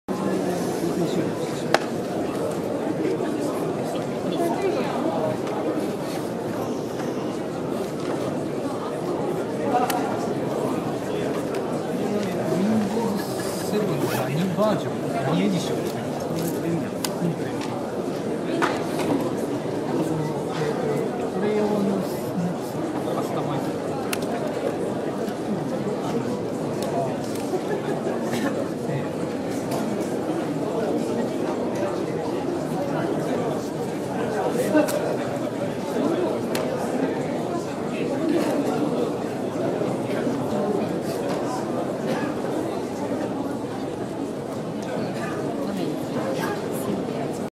F-07C（Windows7モード）でWordを起動
ある意味キワモノな「Windows 7ケータイ F-07C」ムービーレビュー、携帯電話とパソコンが同居する新機軸端末に - GIGAZINE